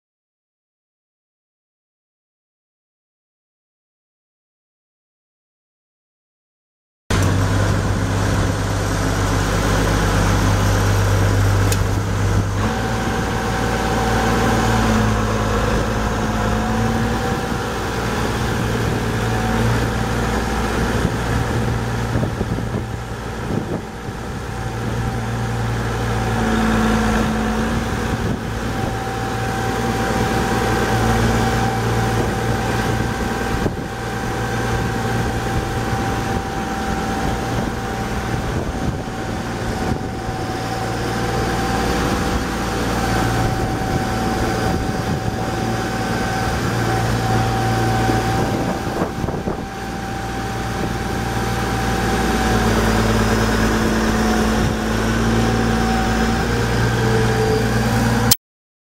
1991 Ford 846 Versatile 4WD tractor for sale | sold at auction December 30, 2013
1991 Ford 846 Versatile 4WD tractor
6,281 hours on meter
Cummins L10 611 C.I.D. six cylinder diesel engine
230 HP
Serial 34643734
Manual transmission
12F-3R gears
AC and heat
AM/FM/cassette
Work lights
Three point
Quick attach
Category 4
Draw bar
(4) rear hydraulic remotes
460/85R38 dual tires
Possible LCD dash display issues
Main center articulating pin replaced November 2013
Serial D475979

It was located in Newark, IL, and we sold it to the highest bidder for $17,050.